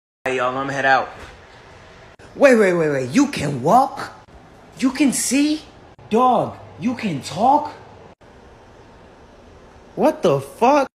You can walk, You can see? (Meme) [Roblox Regretevator Gacha Club] (My AU?)
(Читайте Описание/Read Description)

(ENG):⚠️ATTENTION⚠️: There are a lot of loud sounds in this video, take care of your ears and there is a lot of blood and abusive words and flash thanks for understanding!
(RUS):⚠️ВНИМАНИЕ⚠️:В этом видео там очень много громких звуков берегите уши и там очень много крови и ругательных слов и вспышке спасибо за понимание!

starring/главных ролях:
Spud/Спуд или Как-то на Русском Окучивать
Gnarpy/Гнарпи
Unpleasant/Неприятный
Wallter/Уолтер
Sab/Саб

Apps/Программы:
Gacha Nebula, Roblox, Ibis Paint X, Kinemaster

Time taken/Время:
50 minutes/50 минут